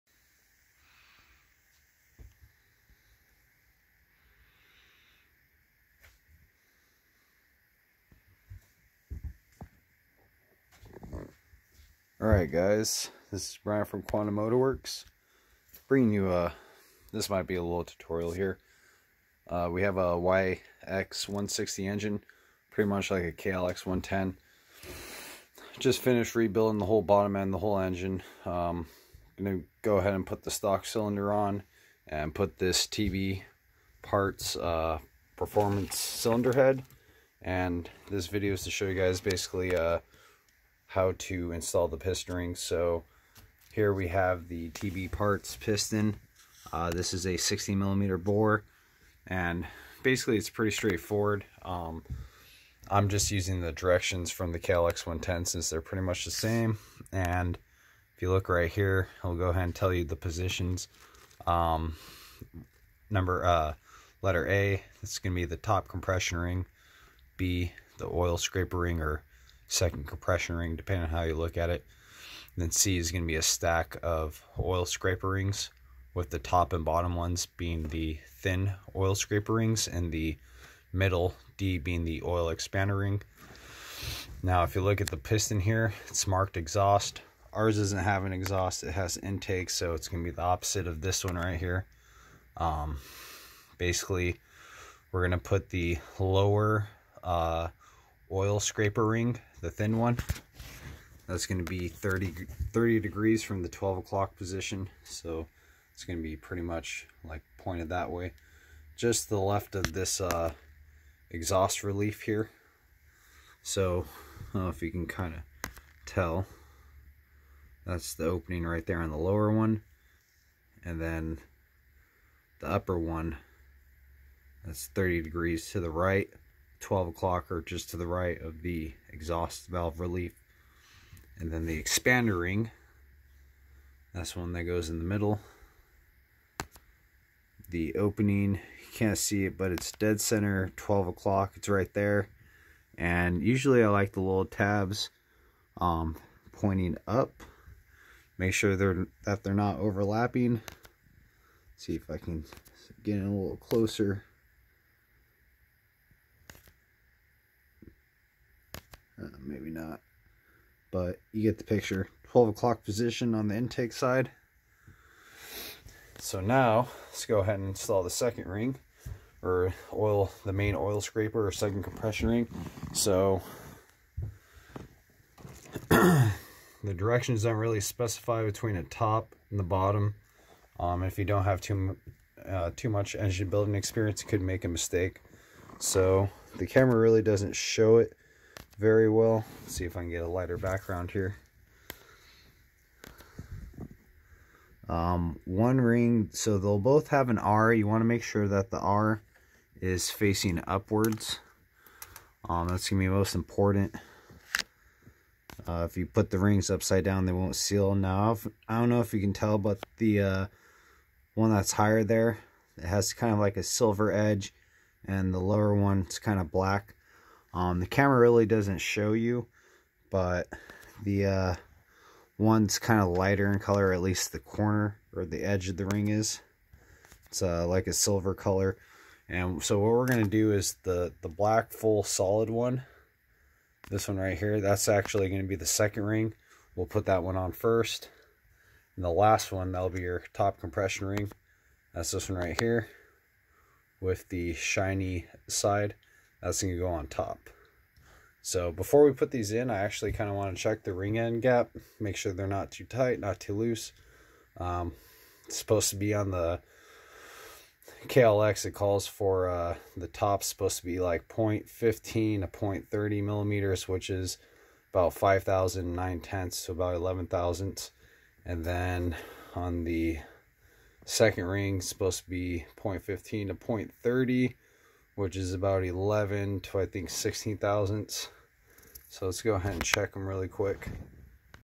SSR160 pit bike engine rebuild with TB parts high compression piston
tb parts 60mm high compression piston installation.